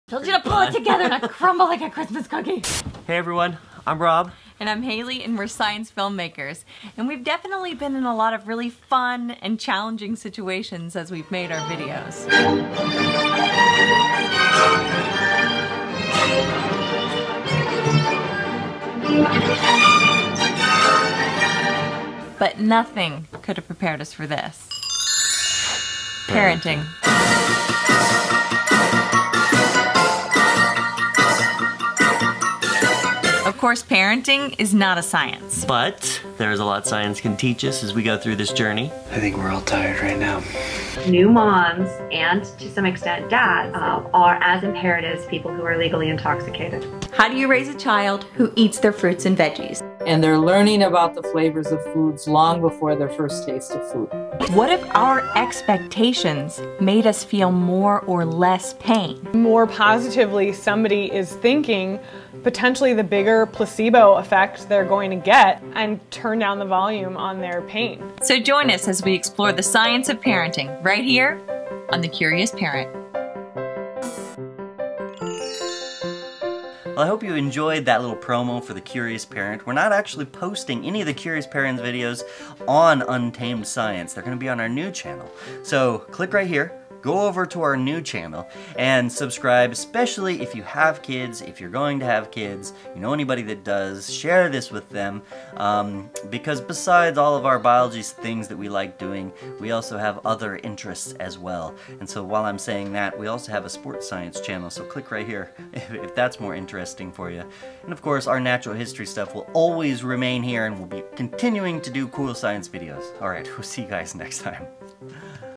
The Curious Parent: Our New Channel!
If you have followed Untamed Science over the last several years, then this set of parent videos makes a lot of sense. If you are coming to the site for the first time, welcome! Here's a little sum-up:

Haley and I are science filmmakers with biology backgrounds. We are in our 30s, making babies, and raising our family. So naturally, we're fascinated and gung-ho about the science of our everyday lives—children, birth, pain, joy, sleep, not sleeping, what our kids eat—basically anything that happens. For us, it's really fun (and sometimes useful or comforting) to ask lots of questions and talk to the top researchers in these fields about the science.